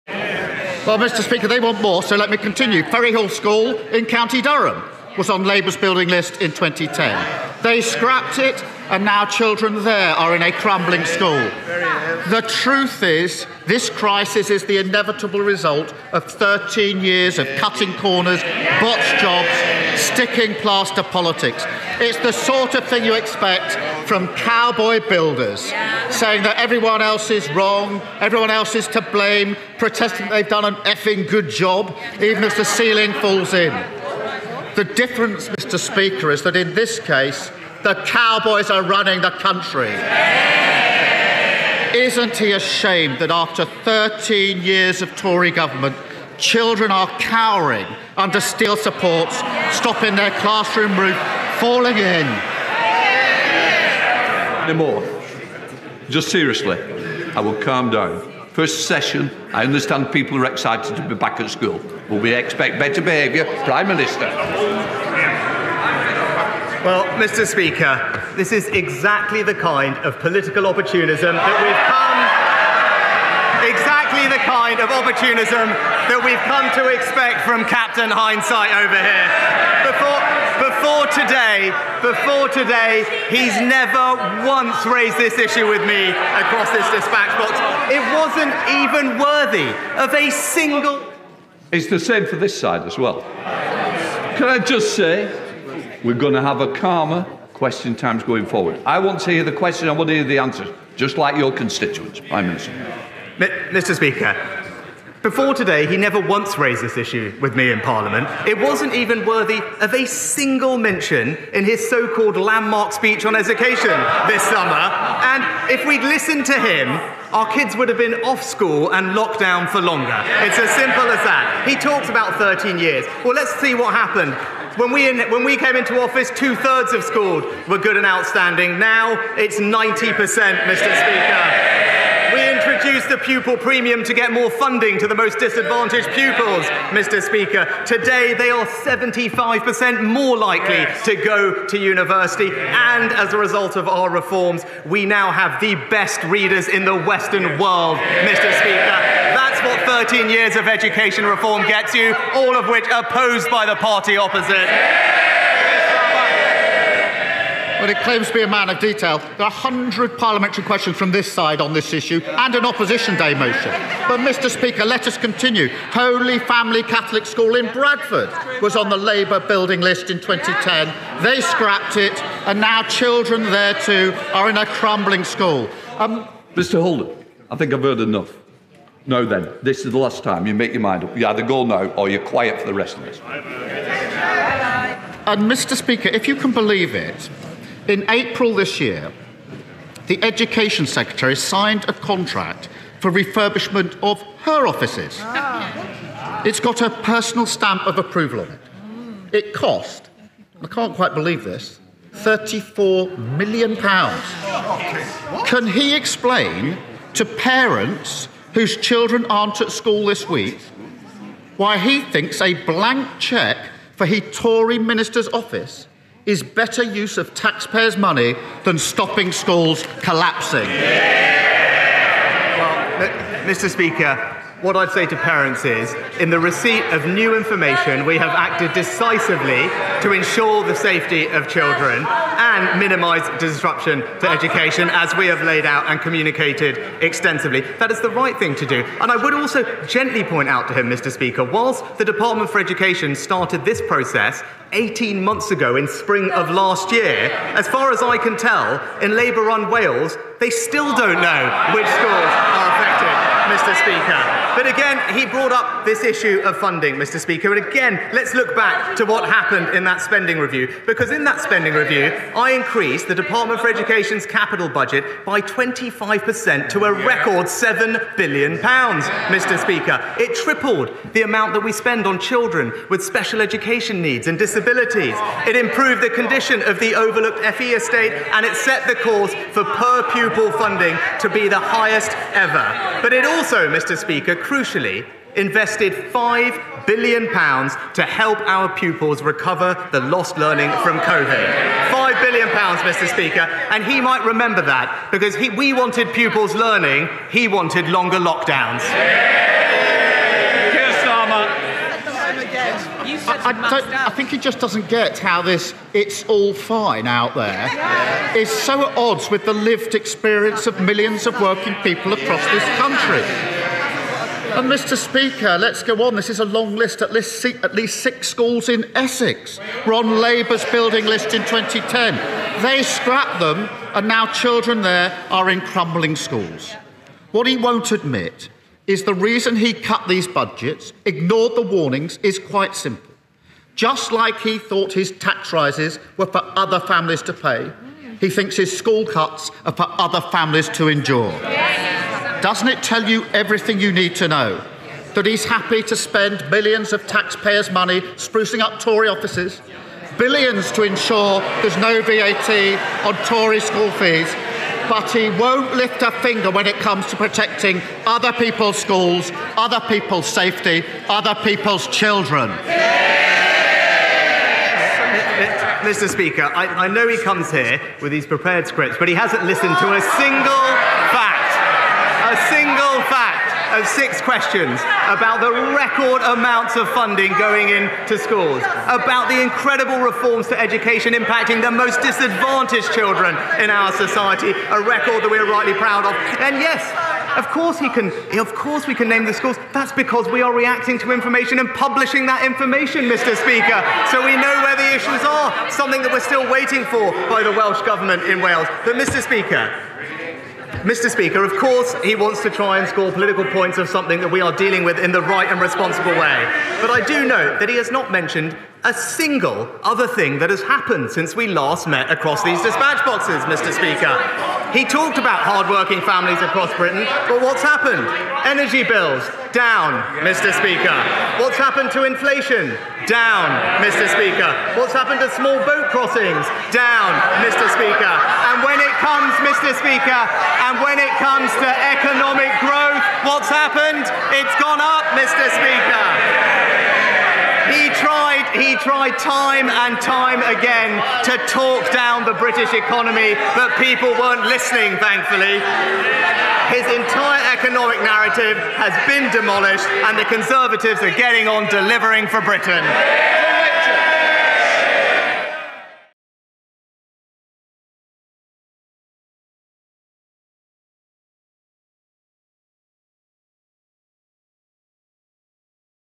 Rishi Sunak attacked over crumbling concrete in schools
Prime Minister Rishi Sunak fires back at Keir Starmer after unsafe schools criticism

Sir Keir Starmer said “the cowboys are running the country” as he took aim at Rishi Sunak over crumbling concrete in schools.

Sir Keir insisted “it’s the sort of thing you expect from cowboy builders”, adding: “The difference is in this case the cowboys are running the country.”

The Prime Minister replied: “Before today, he never once raised this issue with me in Parliament."